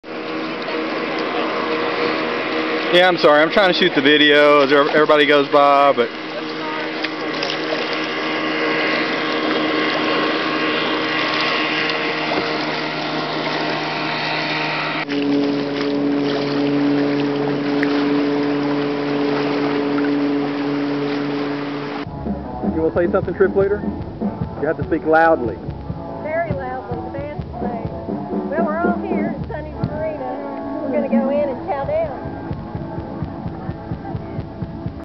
GrillPaddle.avi
Though not a member of the APES (Appalachian Paddling Enthusiasts), I enjoyed joining in for one of the weekly 'Paddling to the Grill' sessions. Nice group of folks!